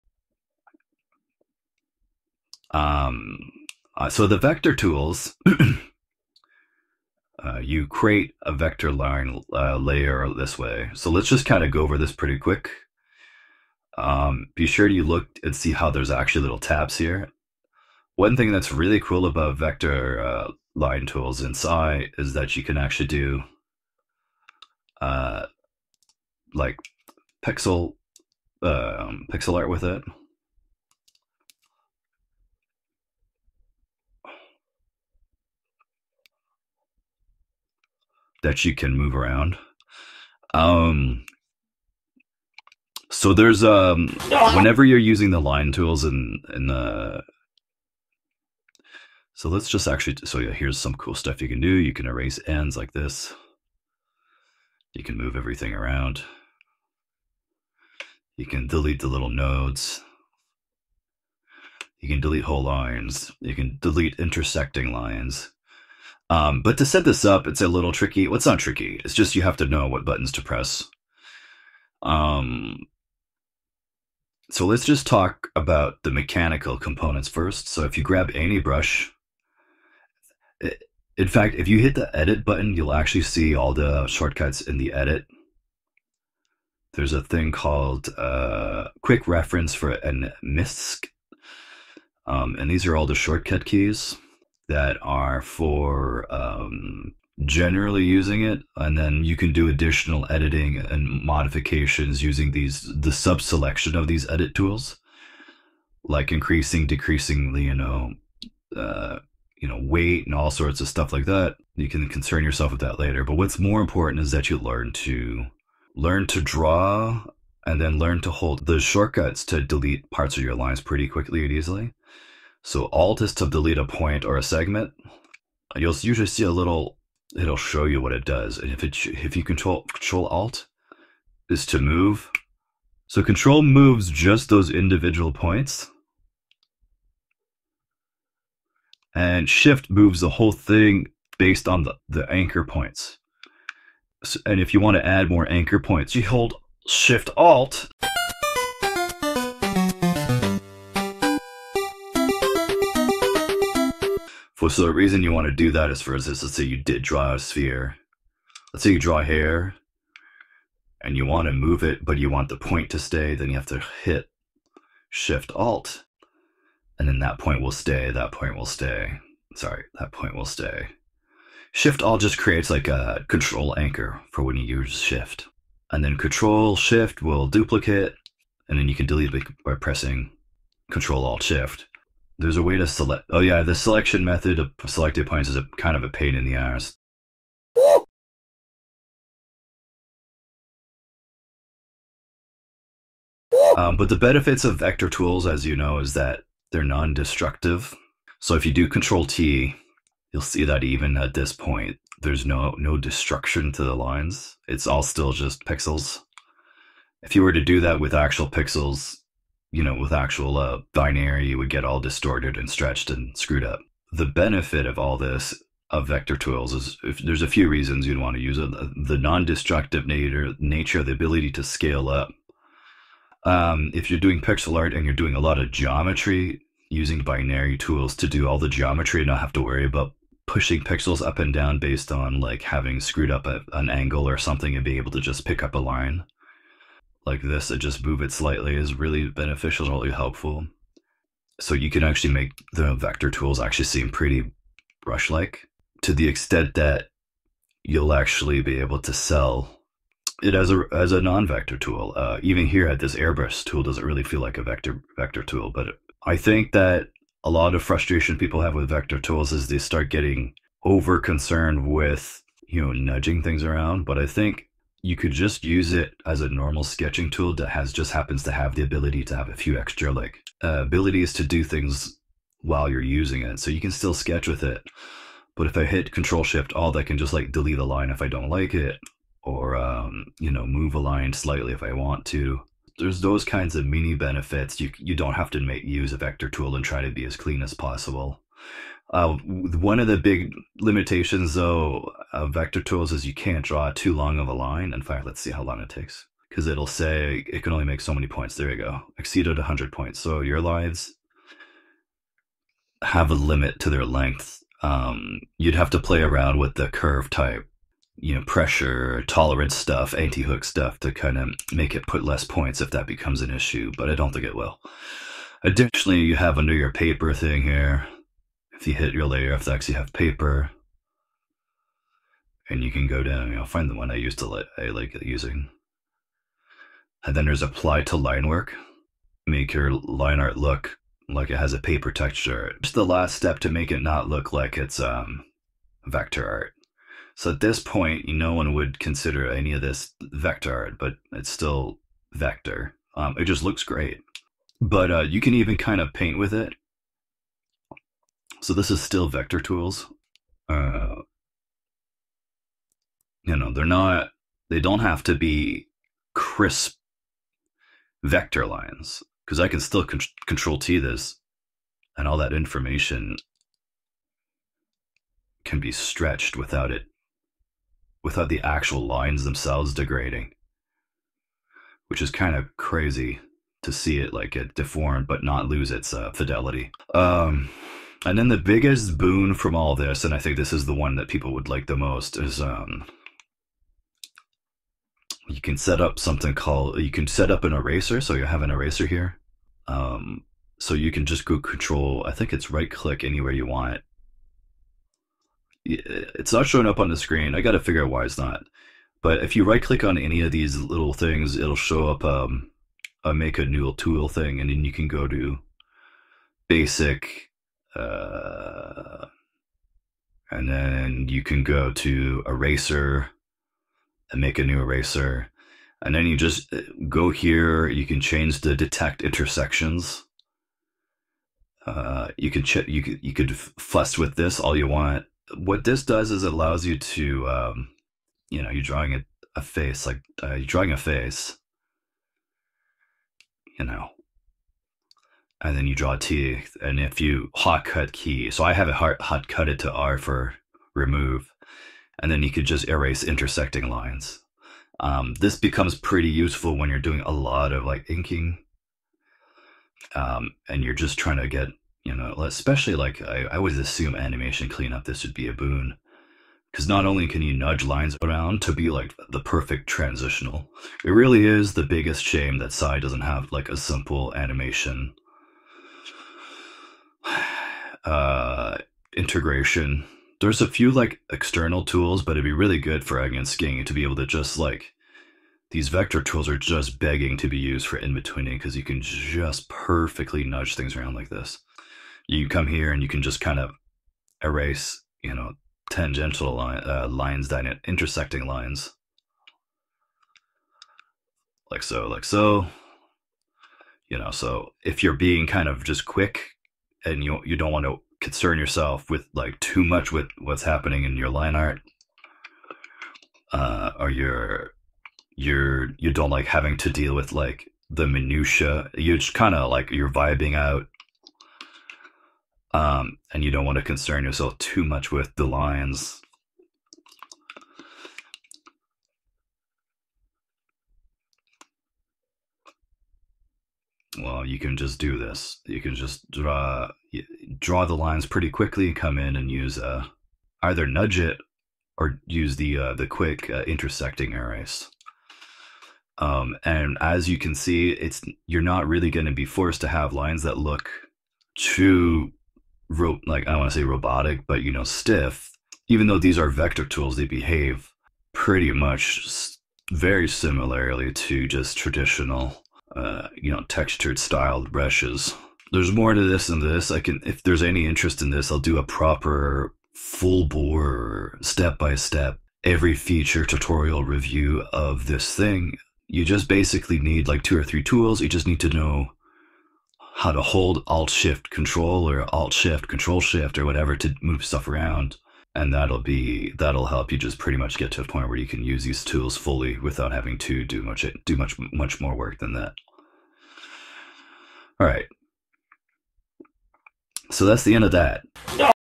Sai 2 Vector Tools | Crash Coourse | Doxytoots
Since I kept telling people how great the Paint Tool Sai 2 vector tools are I felt I should make a short video explaining why they are rad!

* I am using a voice changer because I feel more comfortable that way. That's all.